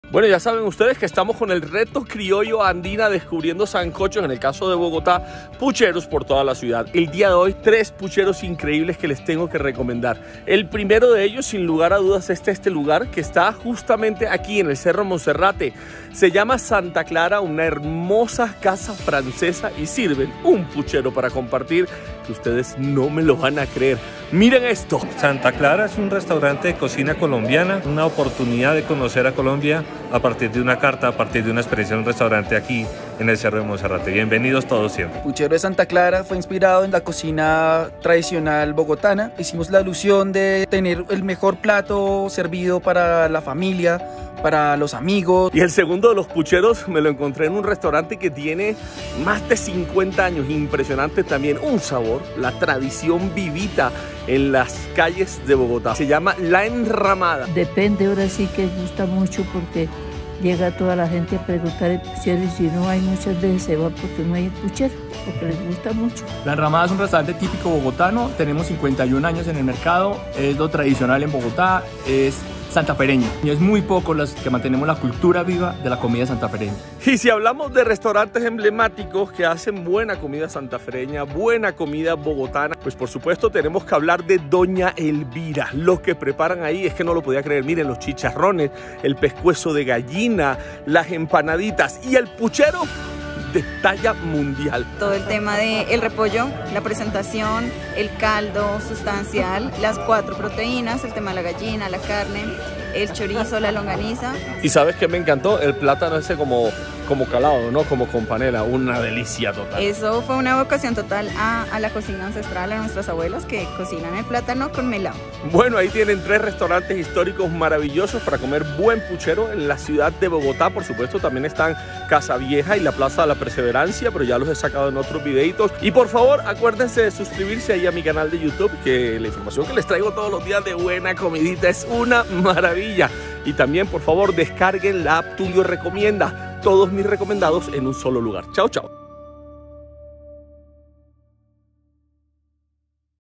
Los 3 SANCOHOS más deliciosos en Bogotá
Ahora... prepárate para esto: ¿Que en BOGOTÁ NO SE COME BUEN SANCOCHO? A ver qué dicen los bogotanos.

Pues sí señores!!! Se llama PUCHERO y es una “PUT... CHERÍA”, jajaja... me encanta su caldo ligero (casi clarificado), y su enorme bastimento servido por allá al lado y, por religion, deben llevar longaniza, arracacha y una hoja de repollo enchumbada en un hogao (además de la gallina, las carnes, la papa, el cilantro, la mazorca de gran pepa y la zanahoria).

Aquí están 3 pucheros de la capital: únicos, diferentes y muy pegados a la tradición... ¿quieres recordar a la abuelita? Pues aquí están:

RESTAURANTE SANTA CLARA
Monserrate

RESTAURANTE LA ENRAMADA
Calle 66 no 17-81

RESTAURANTE DOÑA ELVIRA
Calle 50 no 20-26